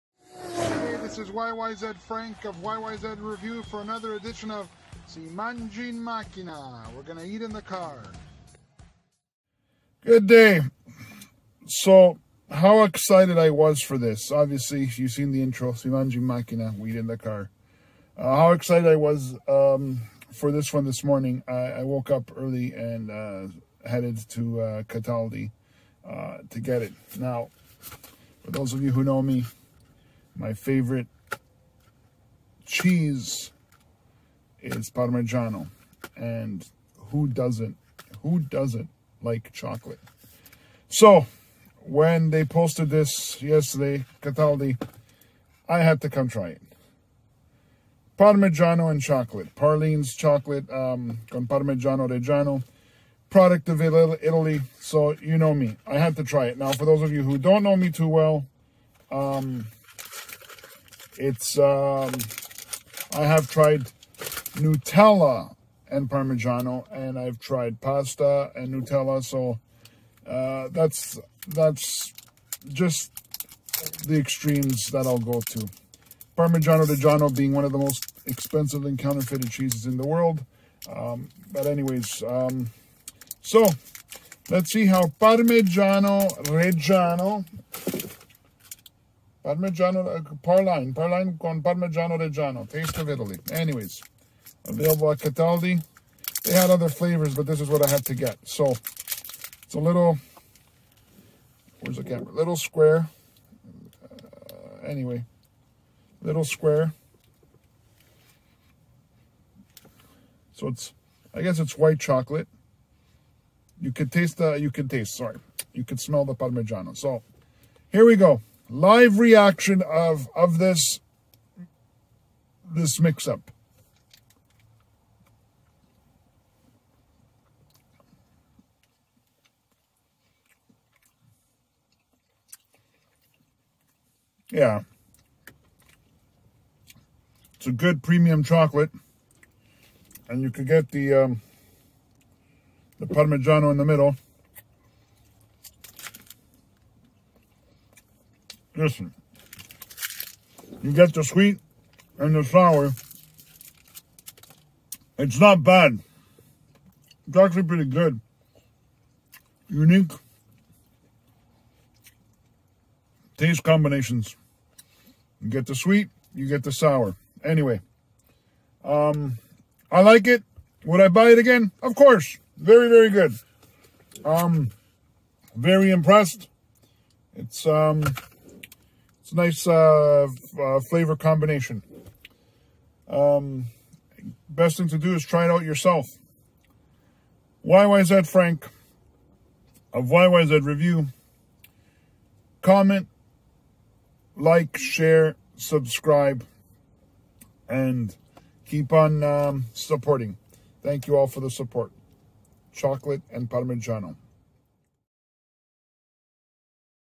PARMIGIANO REGGIANO FLAVORED CHOCOLATE
who doesn't like chocolate and who doesn't like the world's most popular cheese Parmigiano-Reggiano with chocolate it's got a nice sweet and sour taste to it very good something different worth the try